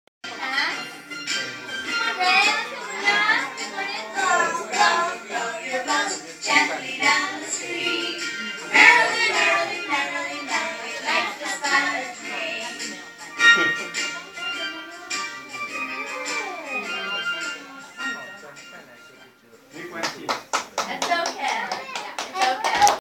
Craig's April 20, 2010 PTA Show Part 10
Craig singing, dancing and playing games at his very first parents and teachers show at Uncle Jasons American School.